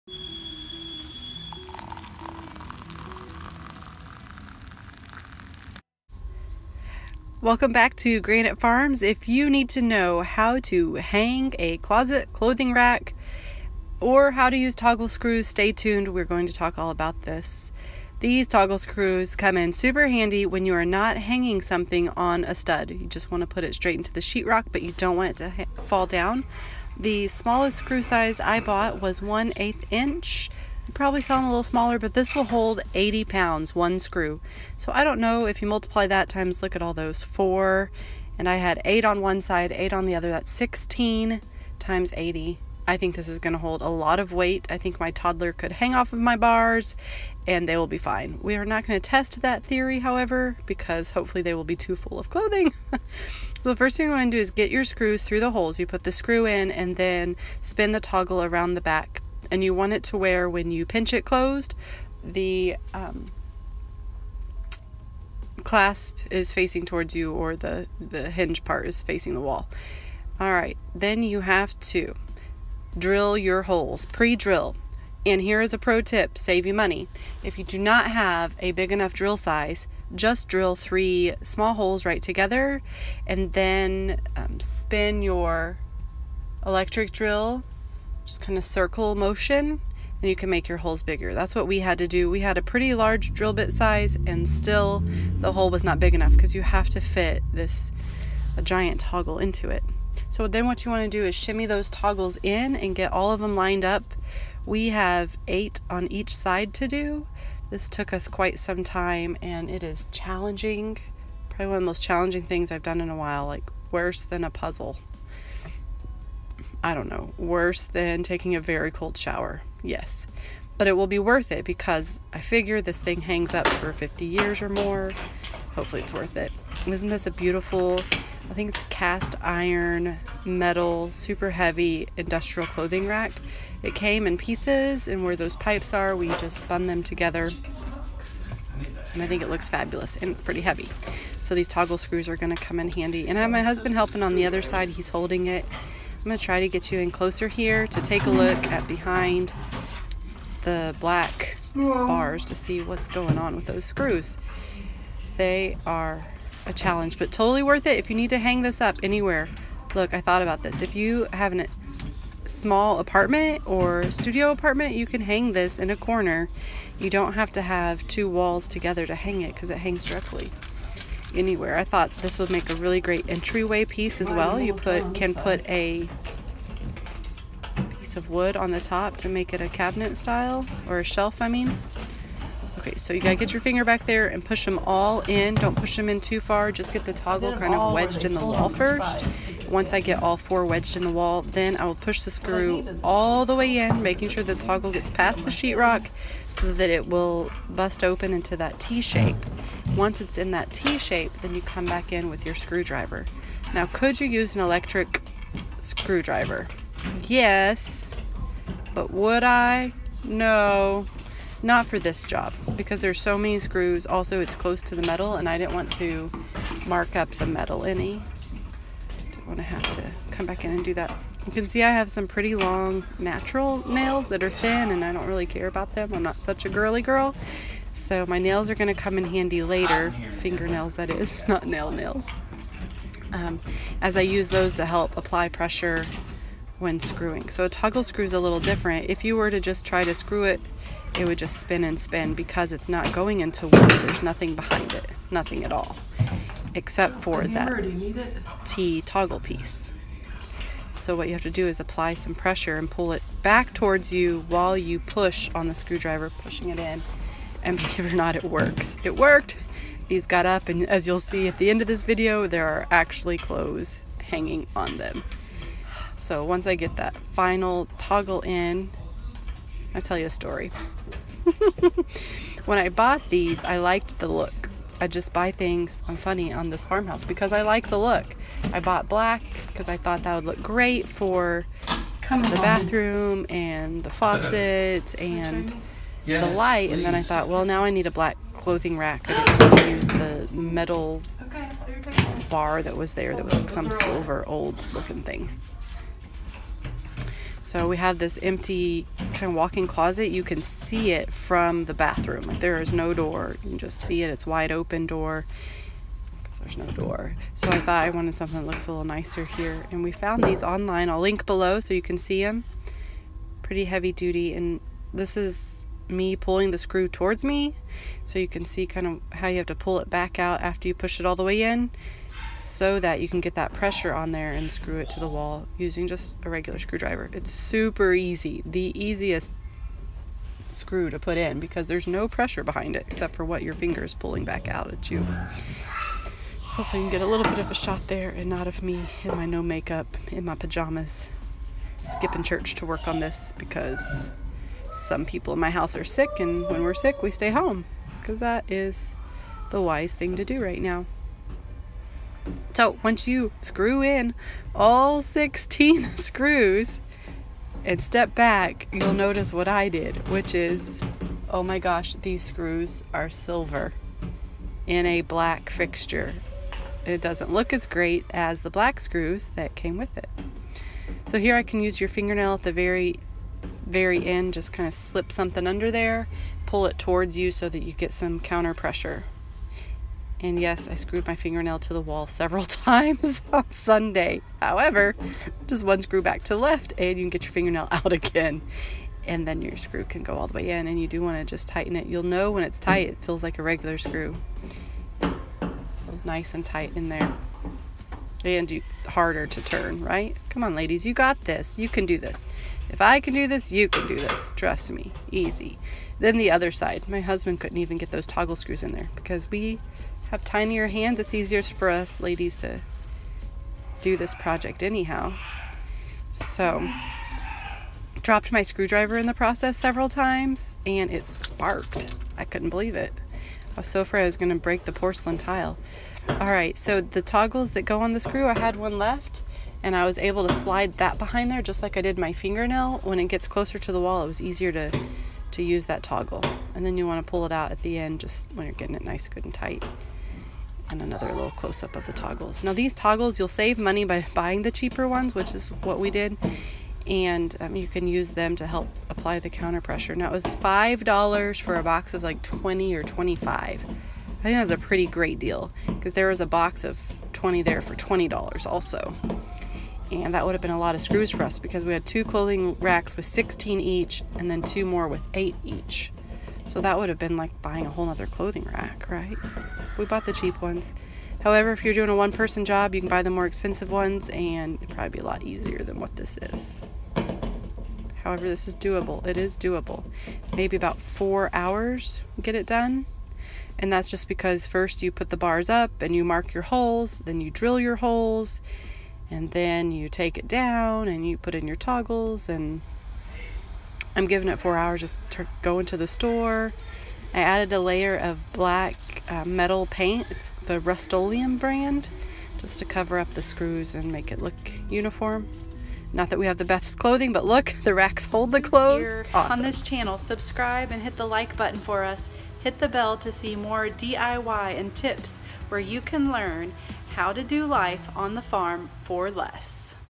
HOW TO INSTAL INDUSTRIAL PIPE CLOTHING RACK. Along with WHAT's A TOGGLE SCREW AND HOW DO I USE IT?
Learn how TOGGLE SCREWS saved my CLOSET REMODEL and HOW TOGGLE SCREWS WORK.  Figuring out small spaces design for our mini walk in closet that has no closet doors, led me to buy this super sleek industrial pipe clothing rack. One CATCH, there were no 2x4 to hang it on, just dry wall. It needed an anchor. Toggle screws were the answer. Now I just need some matching hangers (yes I watched a little HGTV closet first, but this is more about MAKING A SPACE TO HANG CLOTHES where NONE EXISTED BEFORE). STUDIO APARTMENT SPACE SAVING IDEA right here. How'd your project go with toggle screws! These are hard to get started, but an easy fix overall.

OROPY Industrial Pipe Clothes Rack 21.6” Set of 2, Heavy Duty Wall Mounted Black Iron Garment Bar, Multi-Purpose Clothing Hanging Rod for Laundry Room and Closet Storage